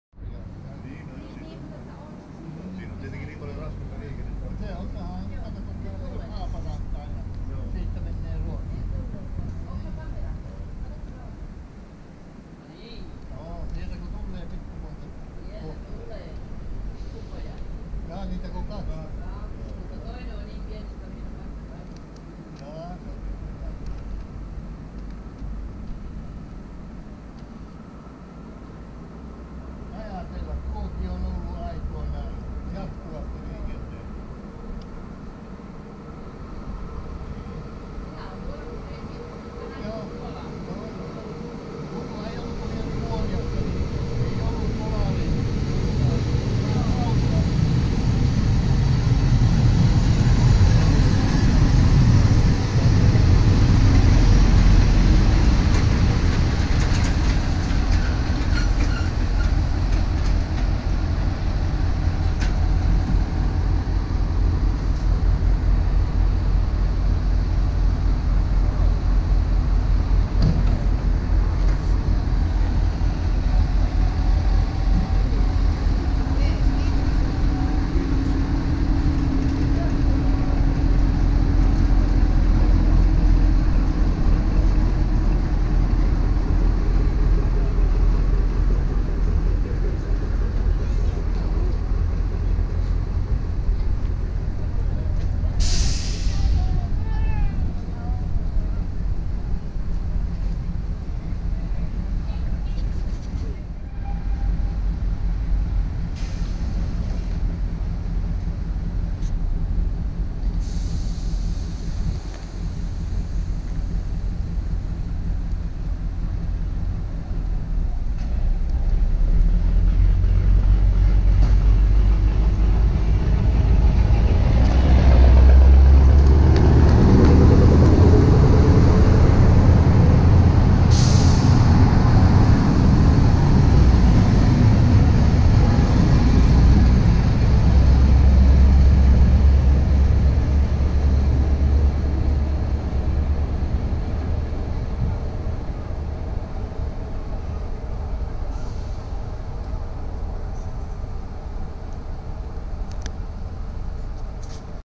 Lättähattu Oulun asemalla 15.6.2019
"Lättähattu" eli kiskobussi vieraili Oulun rautatieasemalla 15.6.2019